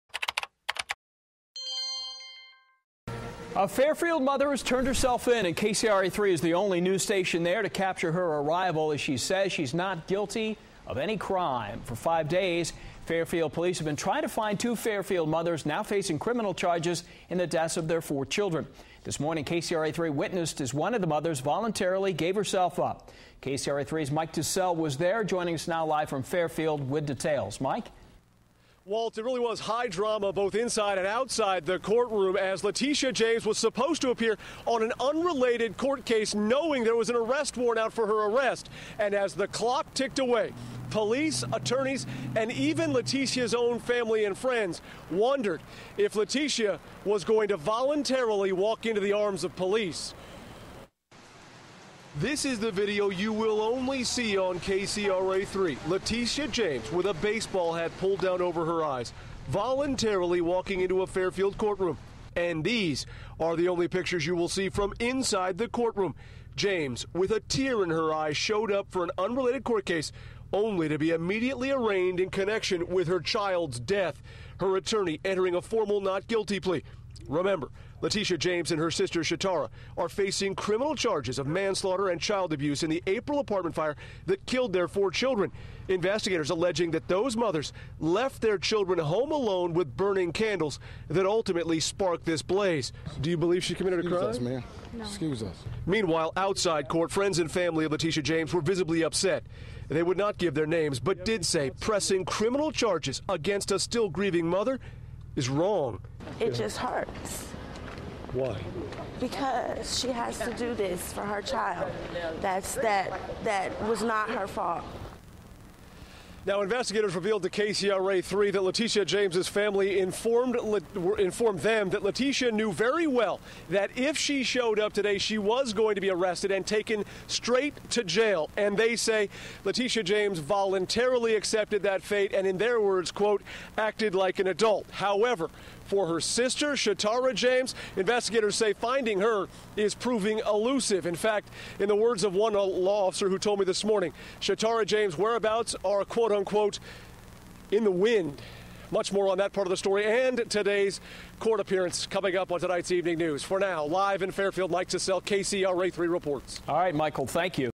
Wanted Fairfield Mom Arrested
Latisha James, a mother whose child was killed in a fatal Fairfield fire, was taken into custody Wednesday in connection with the blaze.